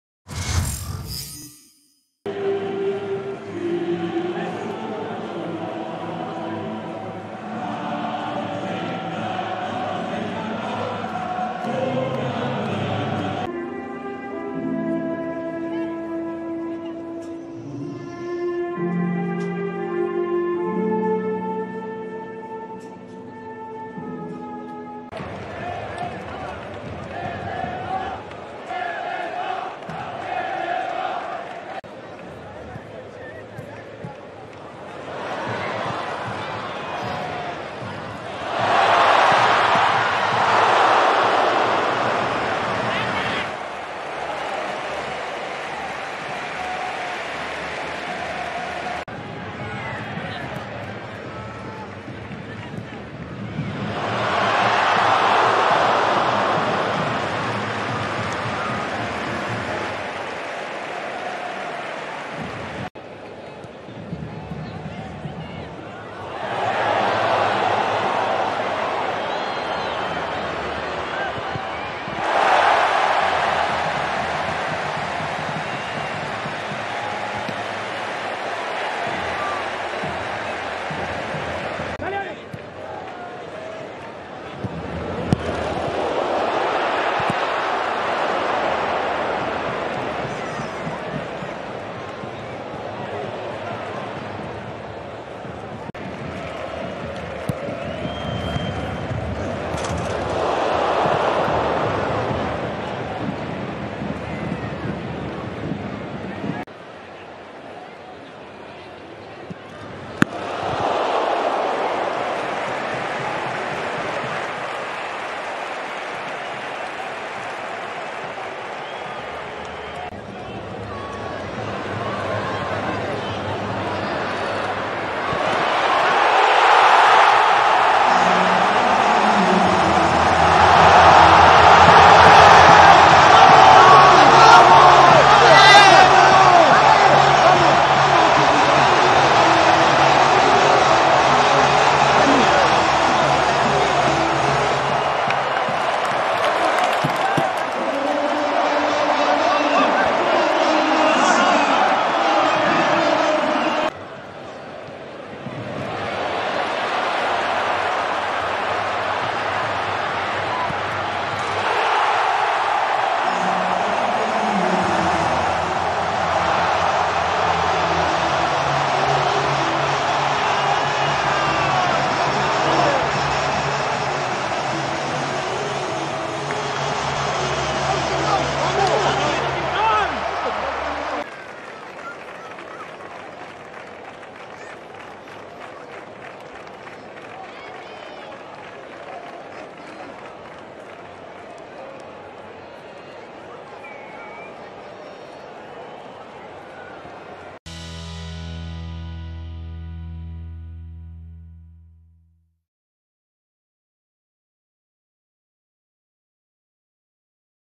BARÇA 2-0 CADIZ | LALIGA 2023/24 MD2
Late goals from Pedri and Ferran Torres earn a thoroughly deserved three points from the first home Liga fixture at Estadi Olímpic Lluís Companys.